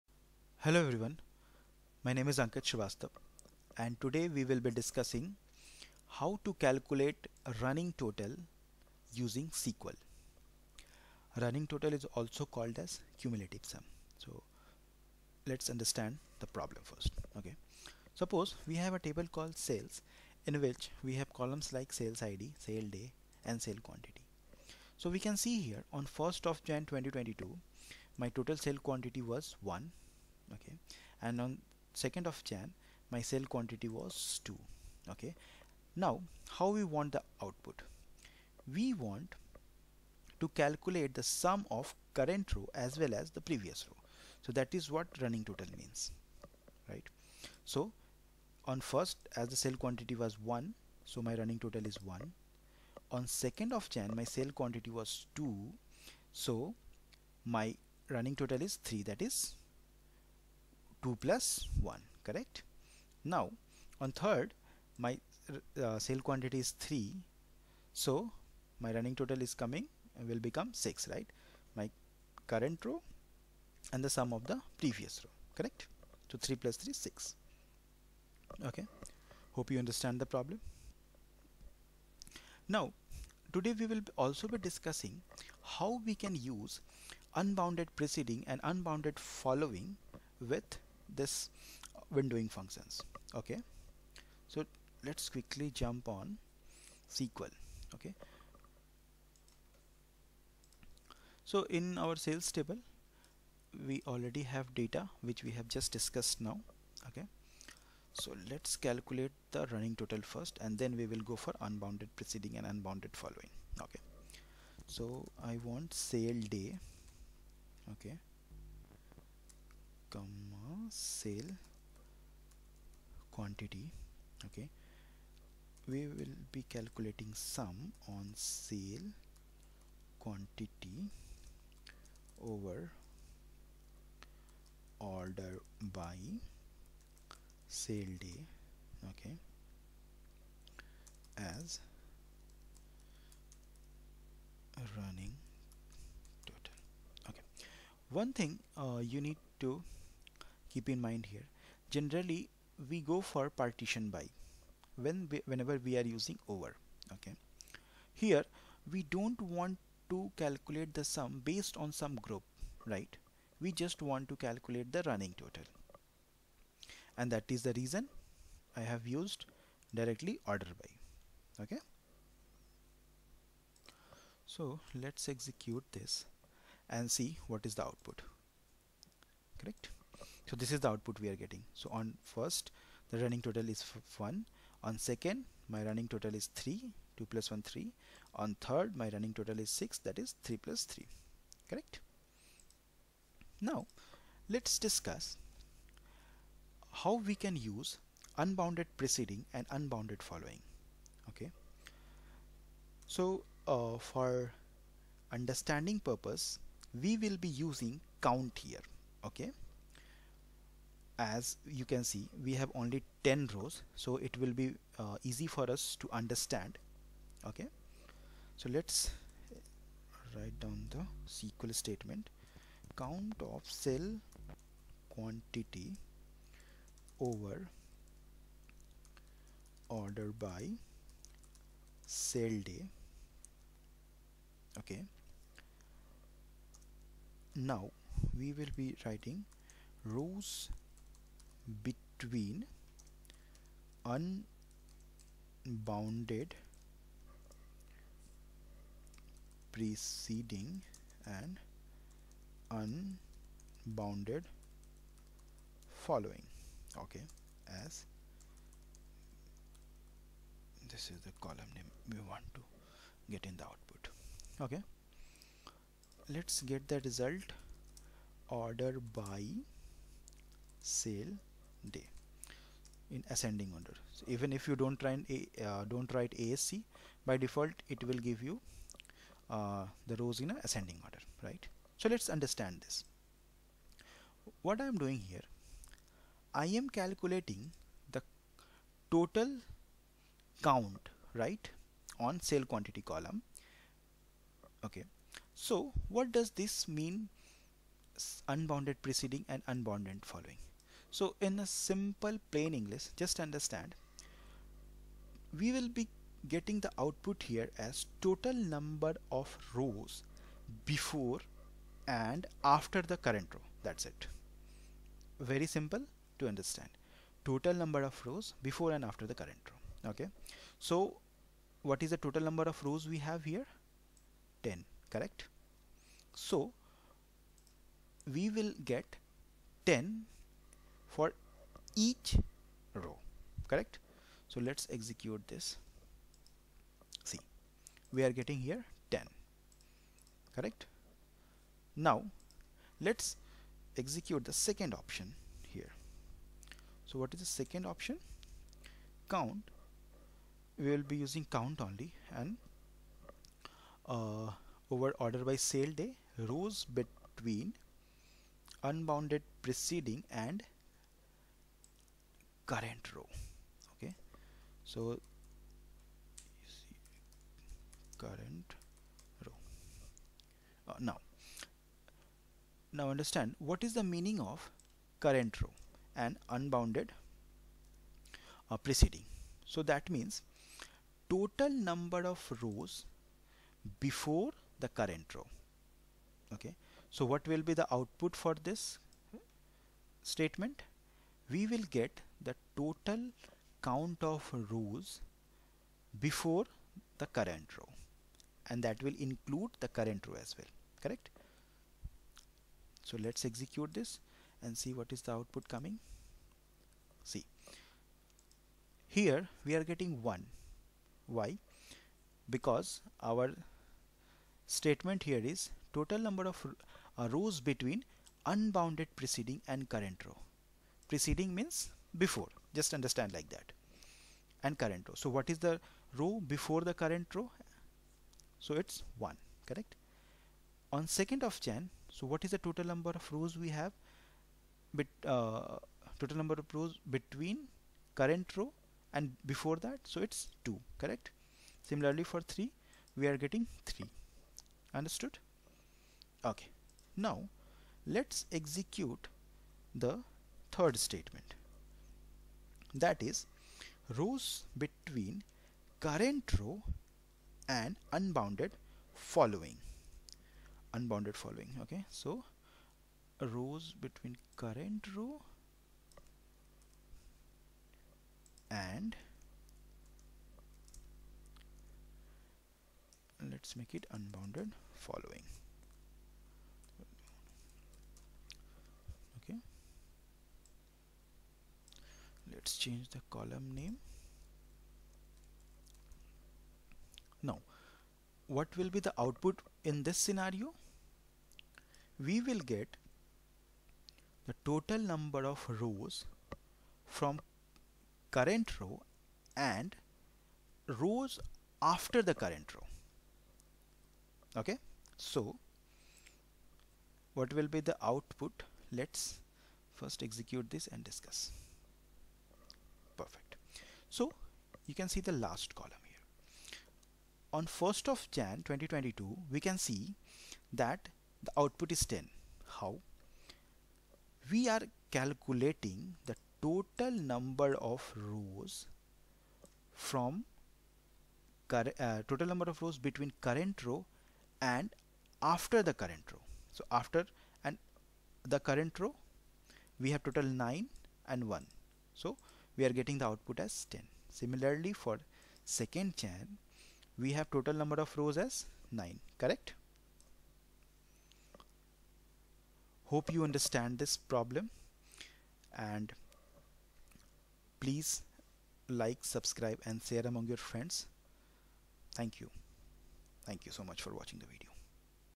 sql Interview Question - Calculate Running Total | Cumulative sum | UNBOUNDED PRECEDING FOLLOWING
Hi All,
My name is Ankit Shrivastava and I am Data Engineer.
Today this vlog is regarding mostly asked
How to calculate Running total/Cumulative Sum using sql .
Also we have discussed how we can use UNBOUNDED PRECEDING AND UNBOUNDED FOLLOWING and  CURRENT ROW with sql window function.

Please find the below DDL we have discussed for practice:-
create table sales

insert into sales values (10,'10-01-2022',10); .